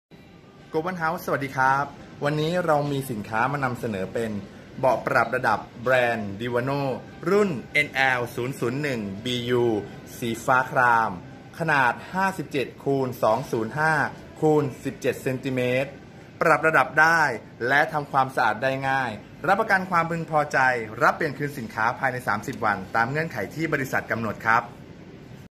Divano เบาะปรับระดับ ขนาด 57x205x17cm NL 001 BU สีฟ้า
ความกว้าง 57.0000 เซนติเมตร
ความยาว 205.0000 เซนติเมตร
ความสูง 17.0000 เซนติเมตร

คุณสมบัติเด่น
เบาะปรับระดับ DIVANO NL-001-BU 57x205x17cm สีฟ้า นุ่มสบาย สามารถปรับระดับเอนพนักพิงได้

จำหน่ายที่โกลบอลเฮ้าส์ทุกสาขา

อินบ็อกซ์ : m.me/Siamglobalhouse
📌 ค้นหาตำแหน่งพร้อมช่องทางติดต่อทุกสาขา